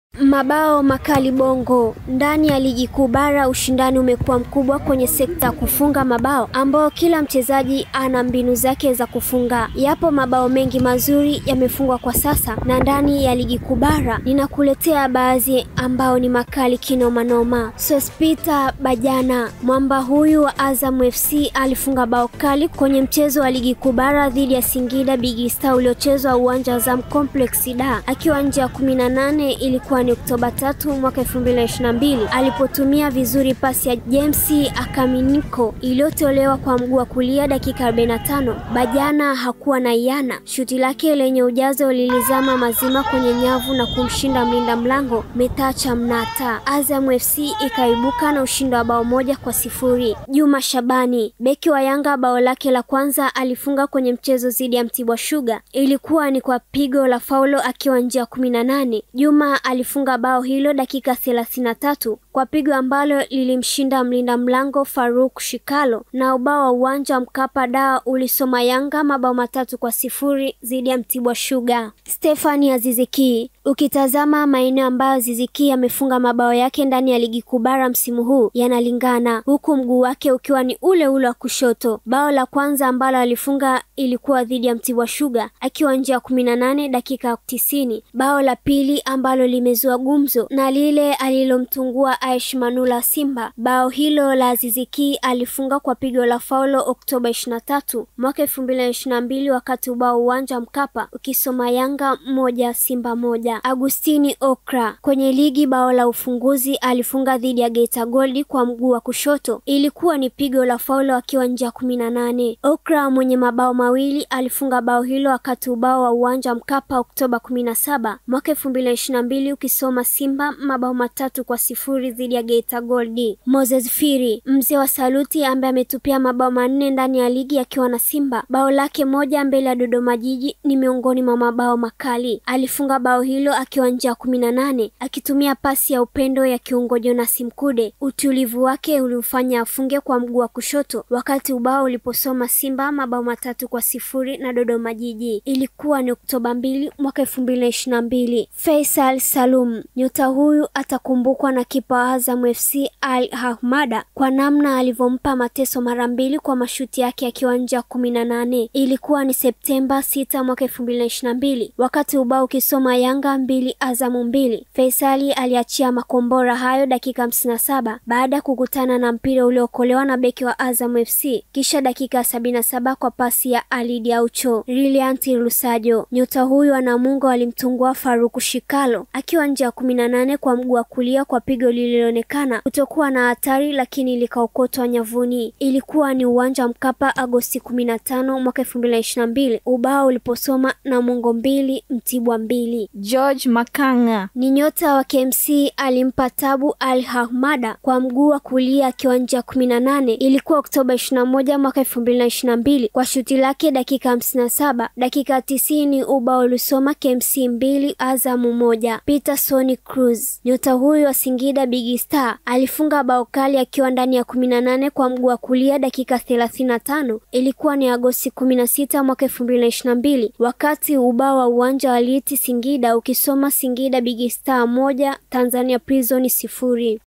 HAYA MABAO MAKALI KINOMANOMA NDANI YA LIGI/AZIZ KI/BAJANA
YAMEFUNGWA mabao mengi makali ndani ya Ligi Kuu Bara ikiwa ni pamoja lile ambalo alifunga Aziz KI ilikuwa mbele ya Simba kwa pigo huru katika mchezo wa Kariakoo Dabi ambapo ulikamilika kwa sare ya bao 1-1.

Pia Relliats Lusajo mali ya Namungo, Moses Phiri naye yumo huku Ali Ahamada na Farouk Shikalo wakiw wametunguliwa mabao mengi makali nje ya 18. .